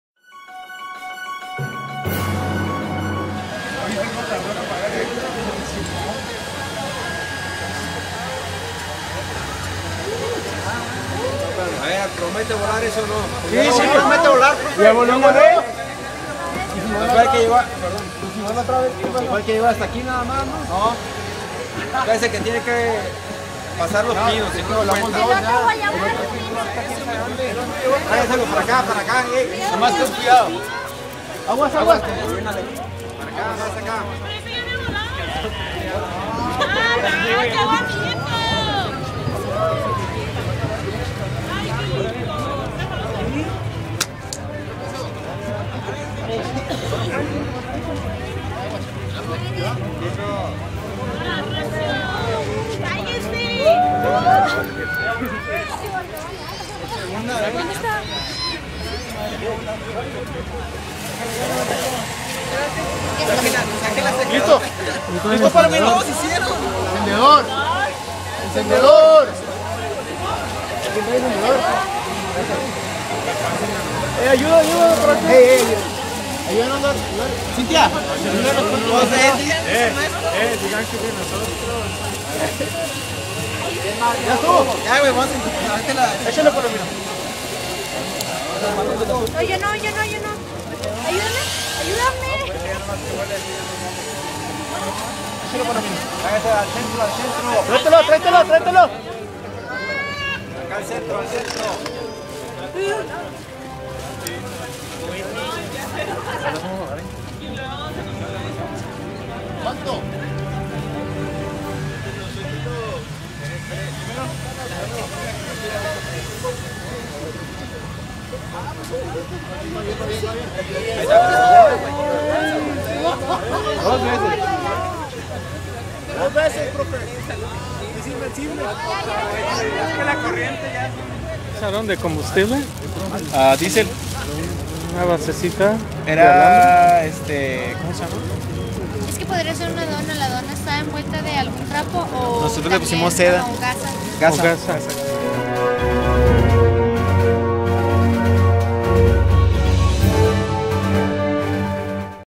Globos Aeroestáticos ITCH 2010
Documental: Proyecto para construir un globo aeroestático, clase del Ing. Roberto Hernández, IT Chihuahua, 2010.